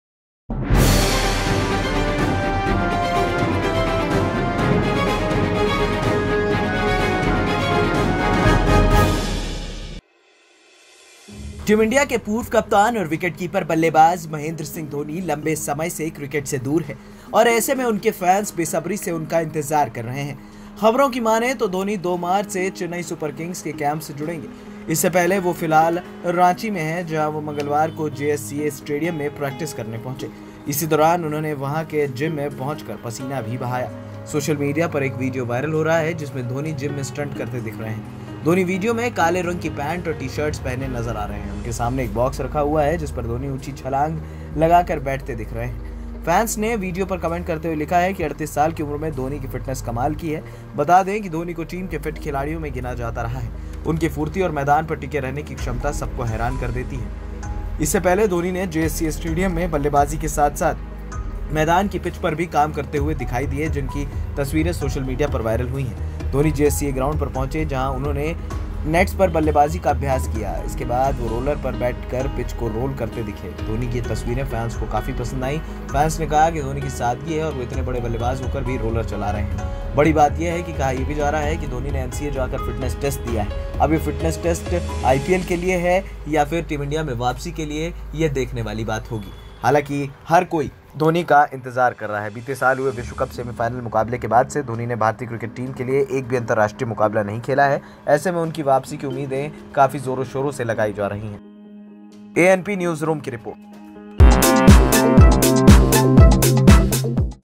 बड़ी खबर!BCCI के कहने पर NCA जाकर धोनी ने दिया खतरनाक फिटनेस टेस्ट,38 की उम्र में दिखा माहि का जलवा
इस ख़ास रिपोर्ट में देखें BCCI के कहने पर NCA जाकर धोनी ने दिया खतरनाक फिटनेस टेस्ट,38 साल की उम्र में दिखा माहि का जलवा ,अब माहि की वापसी कोई नहीं रोक सकता

ANP NEWS
anp news
ANPNEWS
ANPNEWS